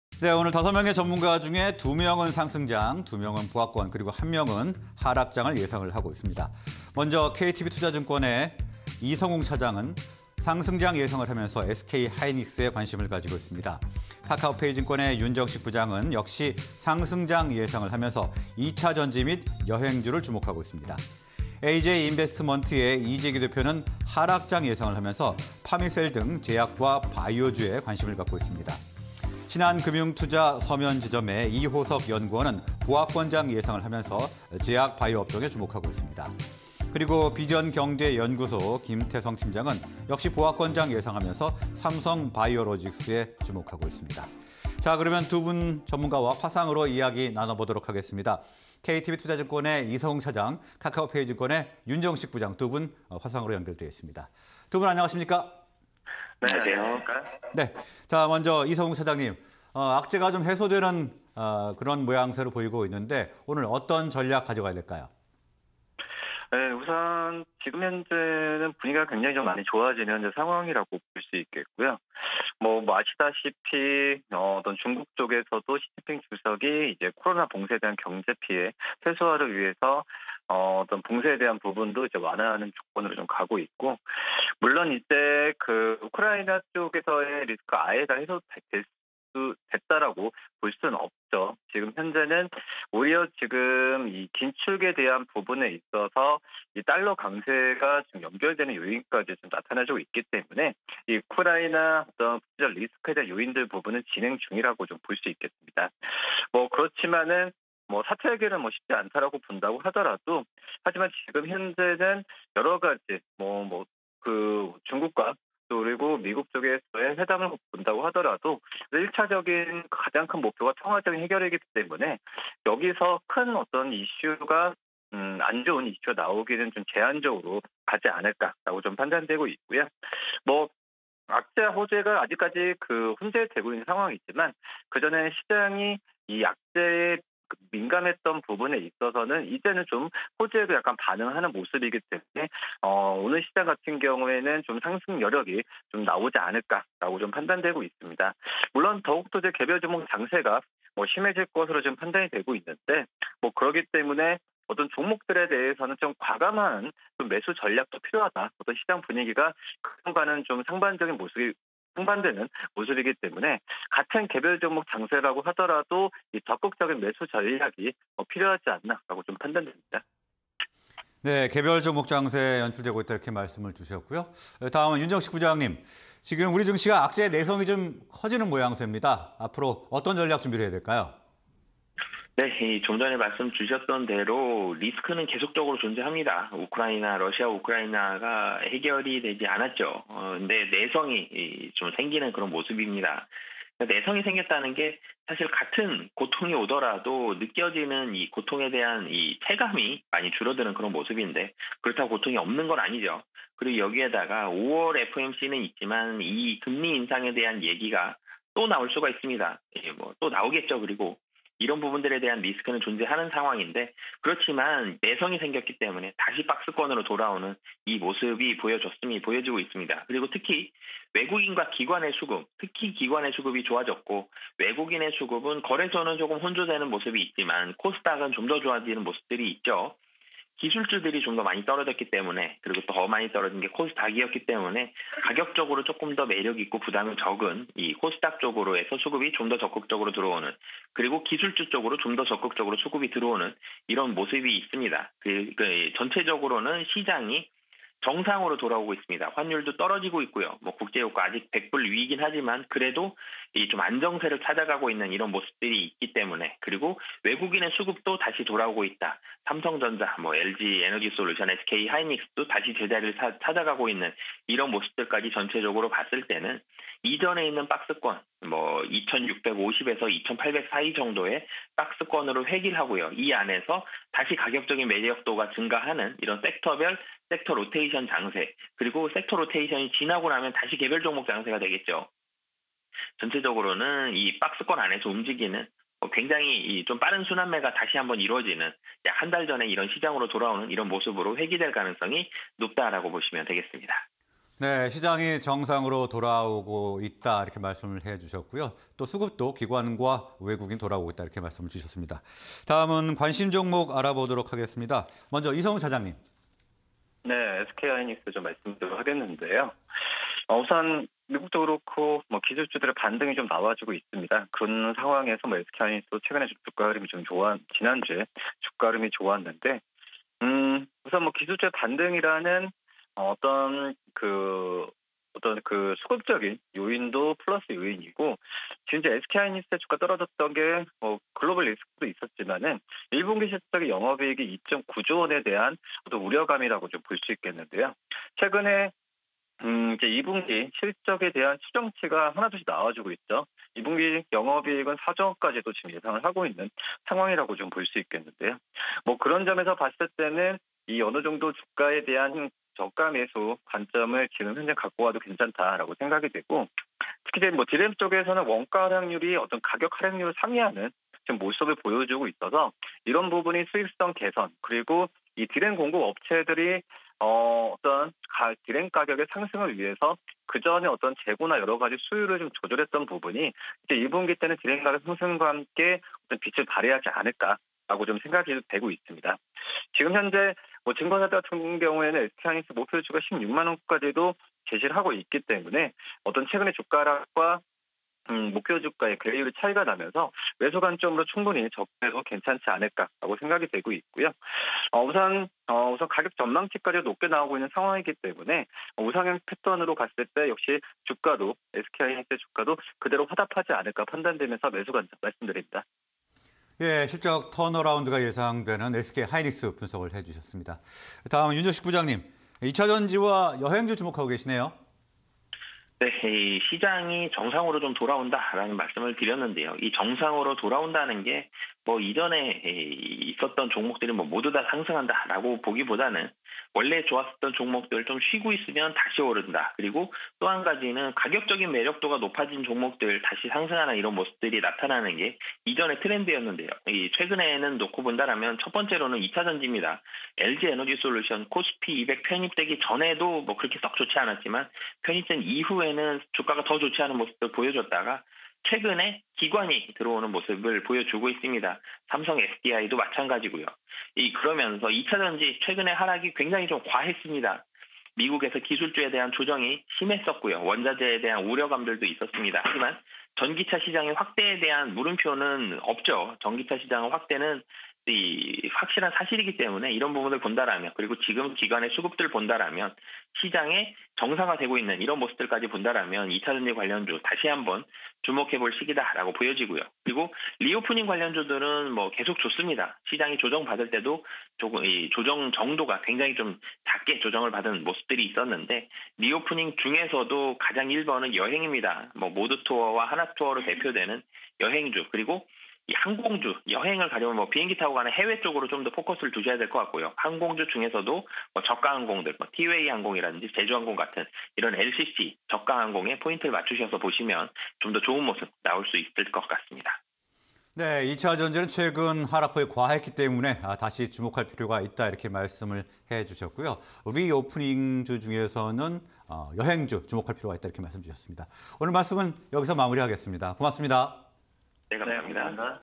SK하이닉스... 증시 불확실성 해소 / 2차전지 및 여행 관련주... 악재에 내성 커진 증시_UP&DOWN 5인의 투자 전략 (20220321)
Q. 증시 불확실성 해소… 반등세 이어가나?
- 불확실성 해소로 지수 상승 기대
- 기술주 반등세… 중국 봉쇄 조치 일부 해제
- SK하이닉스, 실적 턴어라운드 전망

Q. 악재에 내성 커진 증시, 반등 모색할까?
- 우크라이나, 금리 인상 관련 우려 상존
- 외국인·기관 수급 호조
- 박스권 내 제한적 상승 예상
- 2차전지 및 여행 관련주 주목